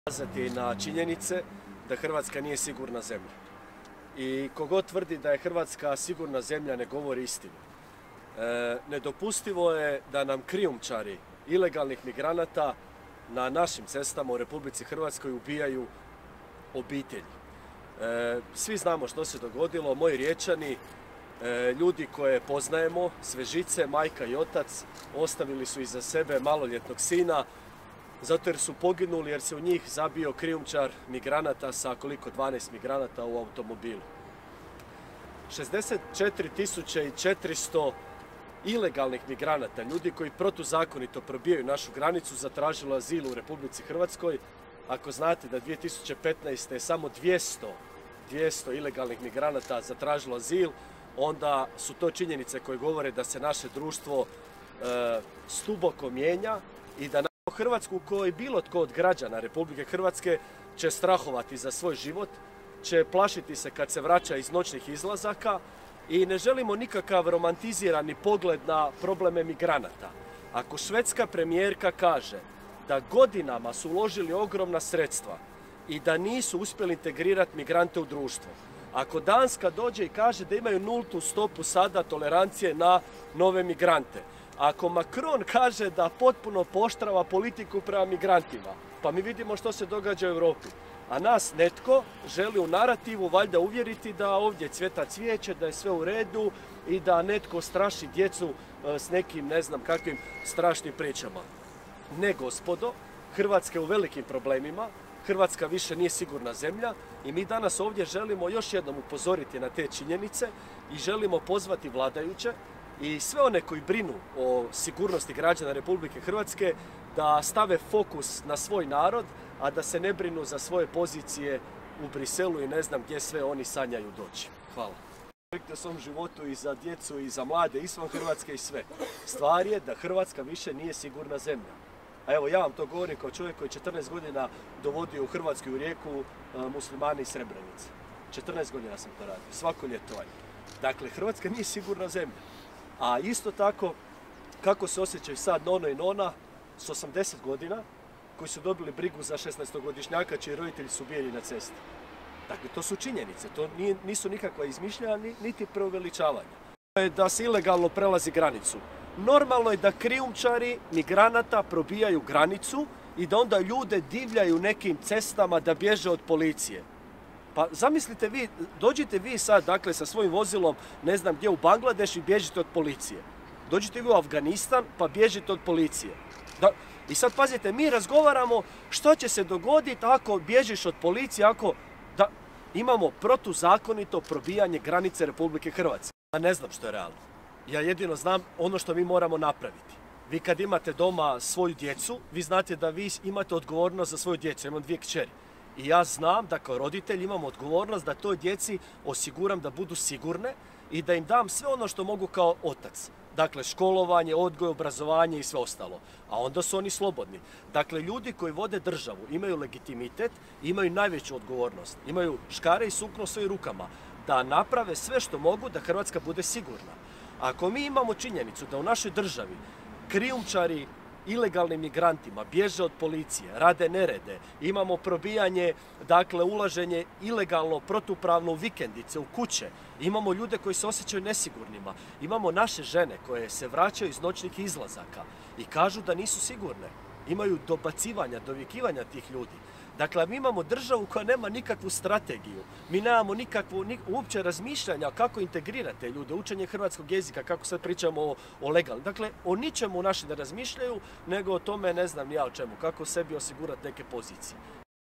Miletić o migrantima: "Hrvatska nije sigurna zemlja"
Saborski zastupnici Mosta Marin Miletić i Zvonimir Troskot, zastupnik Hrvatskih suverenista Marko Milanović Litre te potpredsjednik karlovačkoga Gradskog vijeća Alenko Ribić i načelnik  općine Vojnić Nebojša Andrić održali su konferenciju za novinare o migrantima.